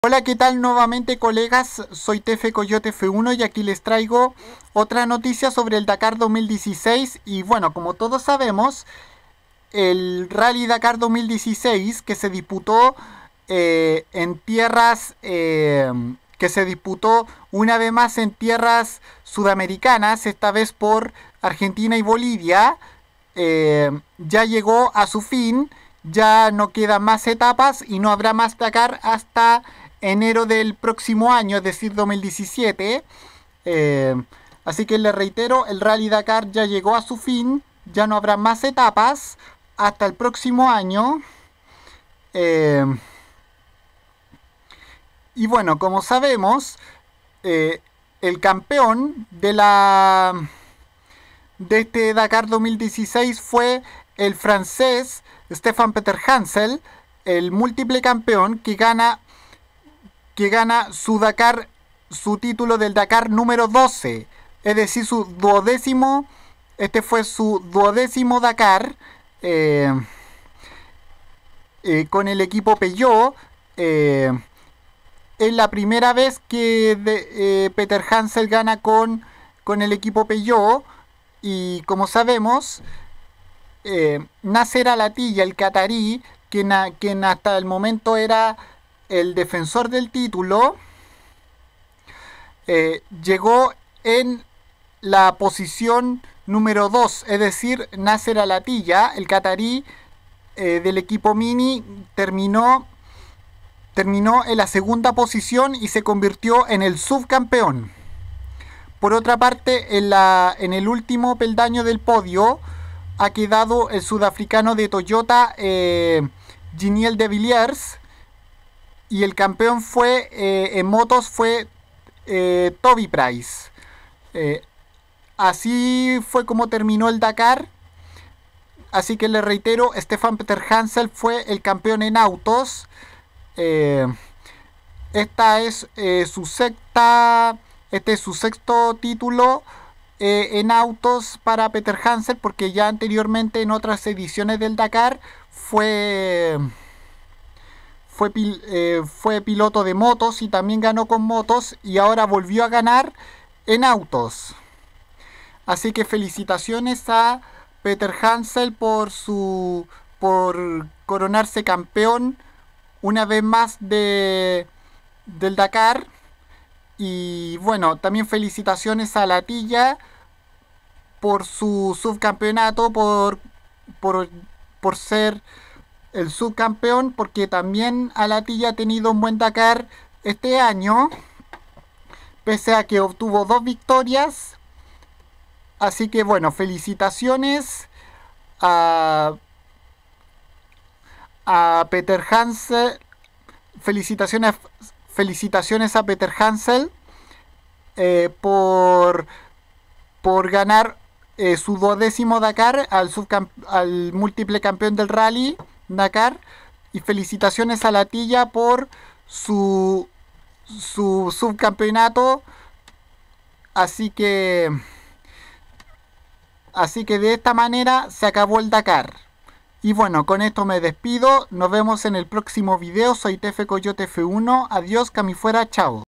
Stephan Peterhansel campeón en autos del Rally Dakar 2016
El piloto francés del equipo Peugeot Stephan Peterhansel consigue su título n° 12 del Rally Dakar 2016,segundo quedó Nasser Al-Attiyah de Mini y 3° Giniel de Villiers de Toyota,en las motos el campeón fue Toby Price